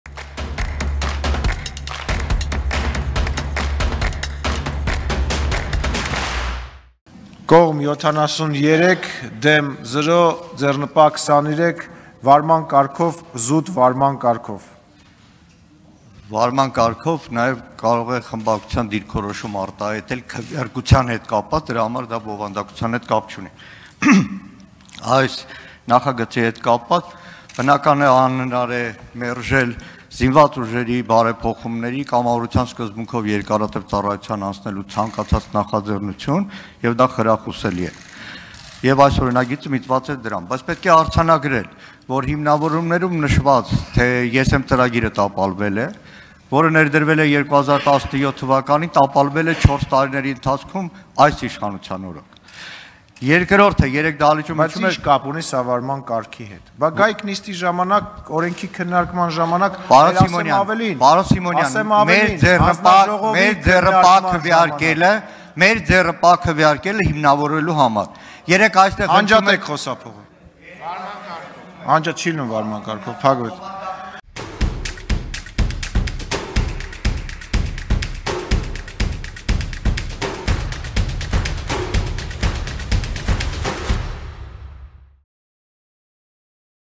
Լեզվակռիվ ԱԺ-ում. «Անջատե´ք խոսափողը». «Դուք տապալե´լ եք «Ես եմ» ծրագիրը»
1in.am Armenian News & Analyses.